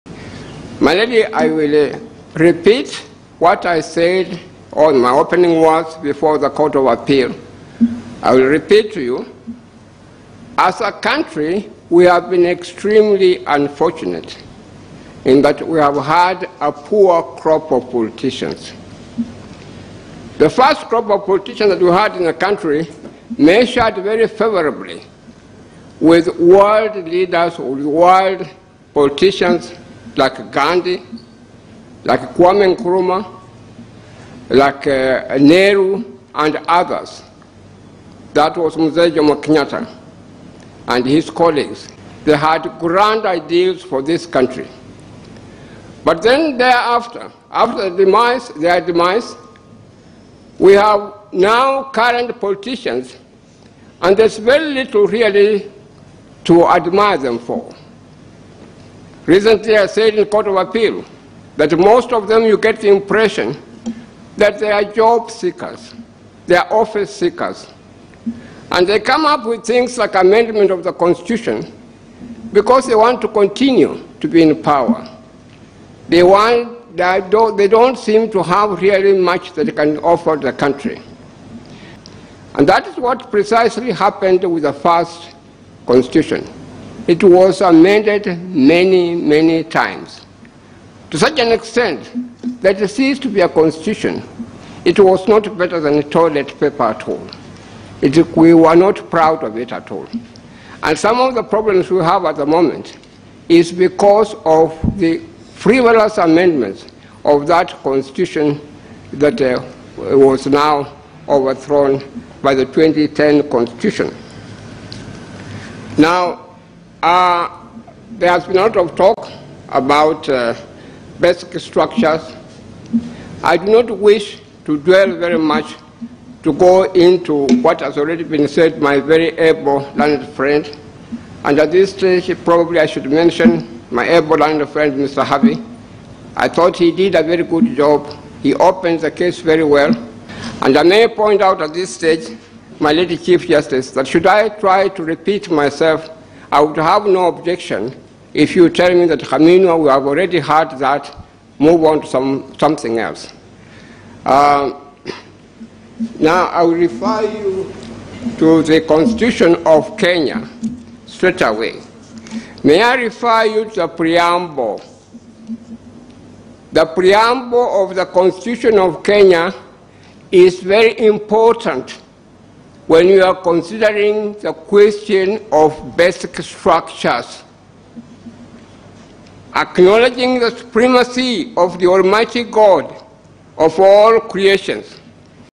BBI Appeal - Lawyer Khaminwa powerful submission at the Supreme Court
Supreme Court hearing of the BBI Appeal submission by John Khaminwa.

Kenya News Today and Citizen TV Live.